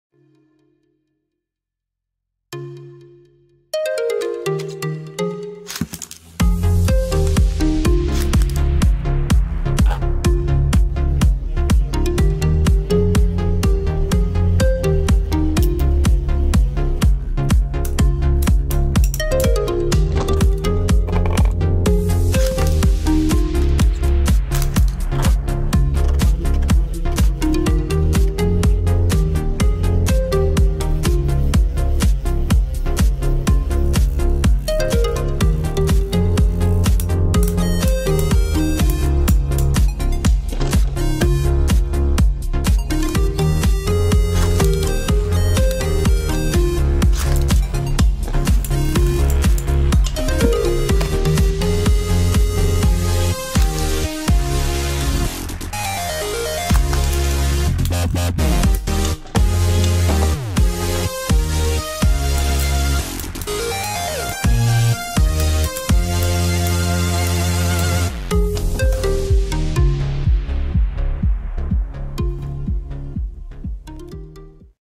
Flash Sagittario 230WD vs Phantom Orion B:D
The Season bladers stamina beys showdown !
Sagittario : Attack mode
Orion : Stamina mode